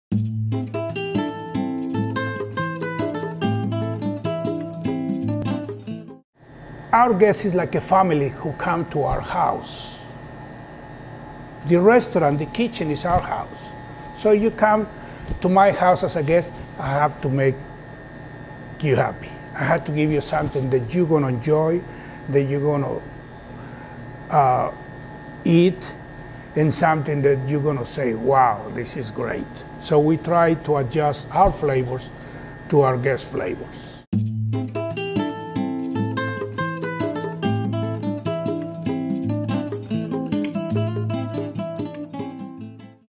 Conversation with Chef, Part 2
Chef Luis is the culinary heart and soul of Abuelo's Mexican Restaurants. Get to know him better in the second installment of a six-part, web-only series.